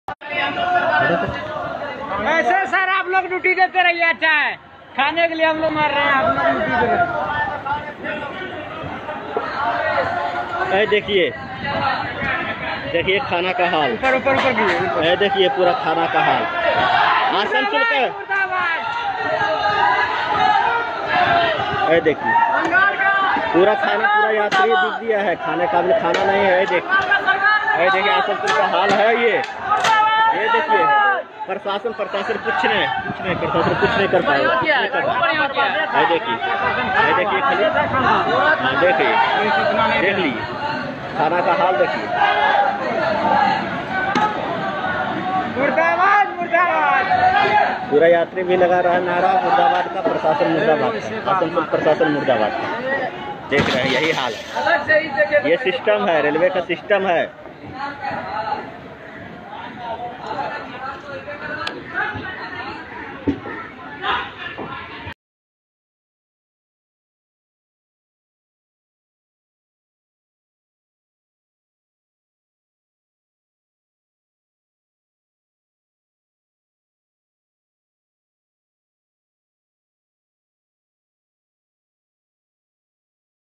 Travelers Reaction while Leaving the Railway Station in the Shramik Train during the Lockdown Period
Please Subscribe to my YouTube Channel and Watch Like Subscribe and Share this Video about the Travelers Reaction while Leaving the Railway Station in the Shramik Train during the Lockdown Period